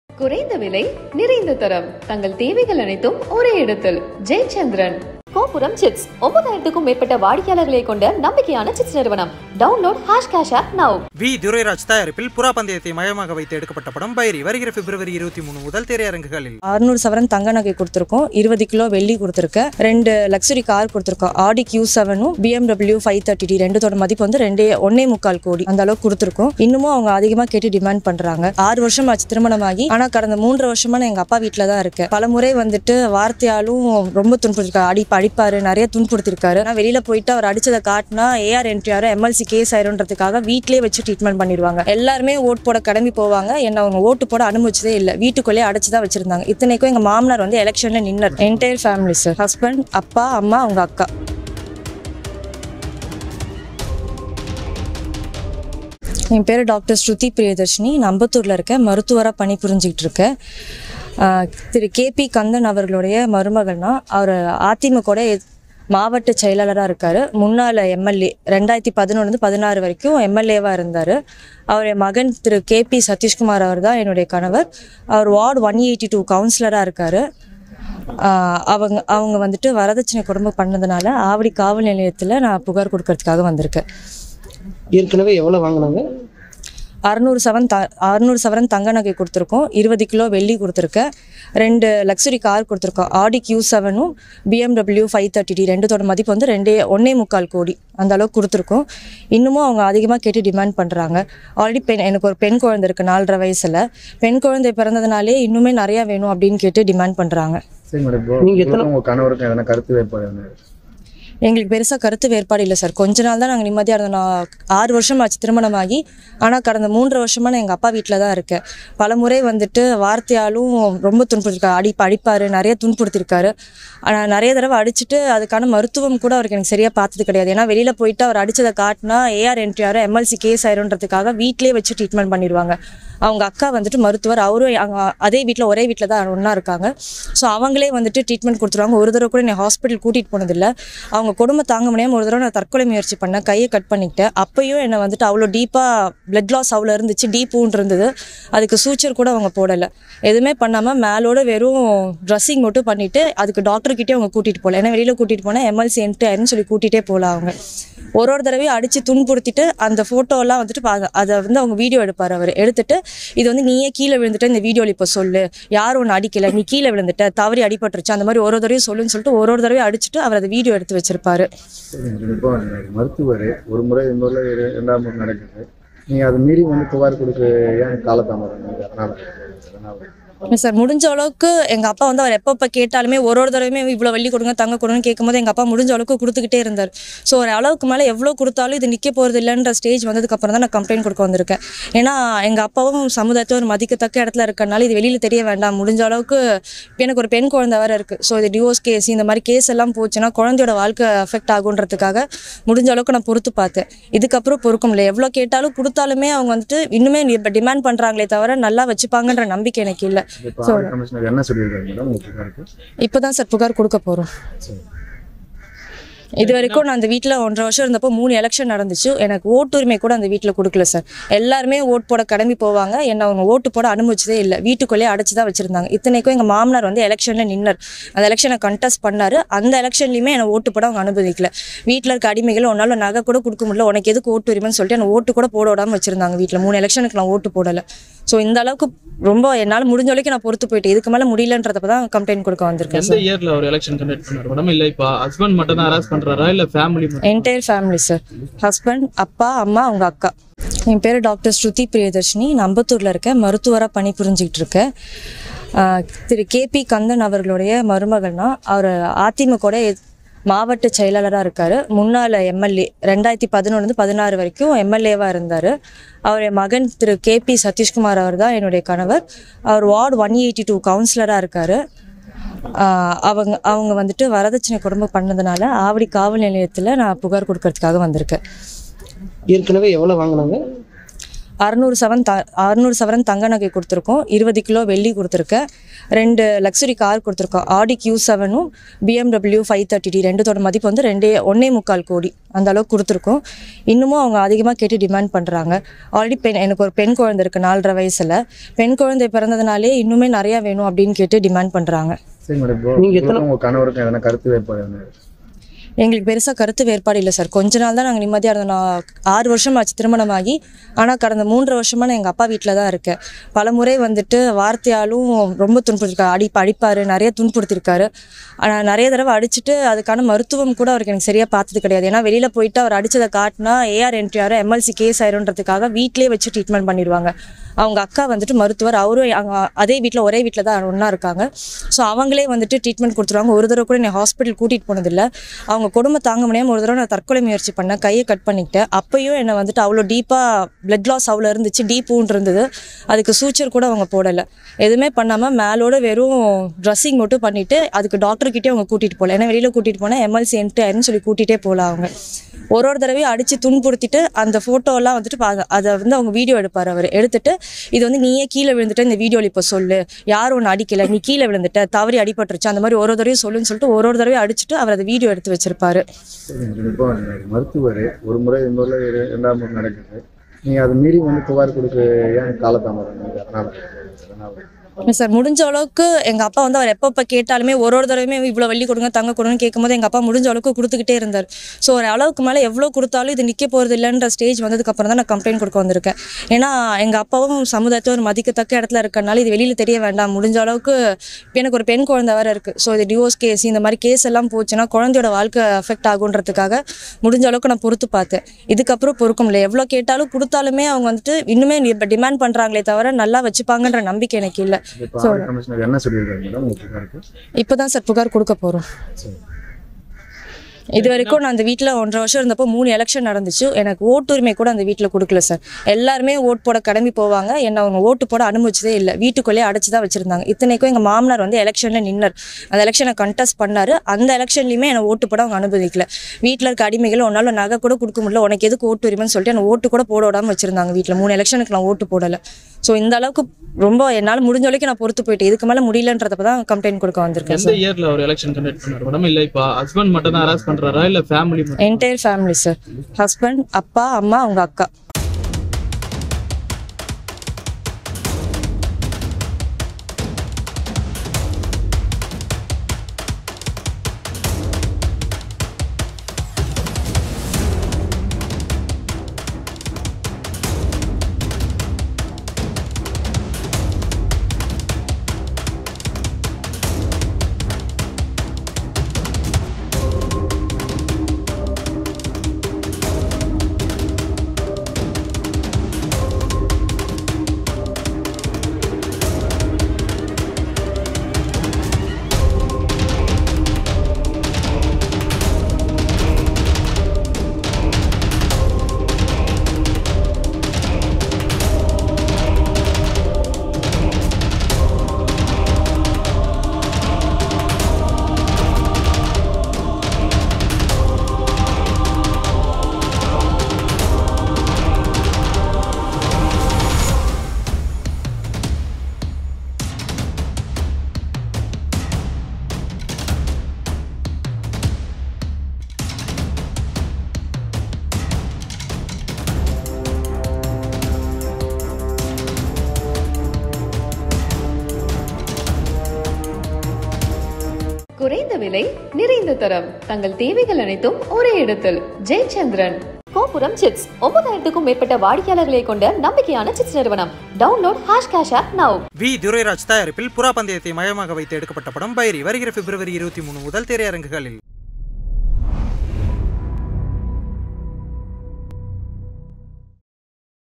கொடுமை தாங்கமுடியல... ADMK Ex MLA KP Kandan மீது மருமகள் புகார் | EPS | Sholinganallur | Dowry | IBC
கொடுமை தாங்கமுடியல... ADMK Ex MLA KP Kandan மீது புகார் | EPS | Sholinganallur | Dowry Case | IBC Tamil

Kopuram Chits Private Ltd.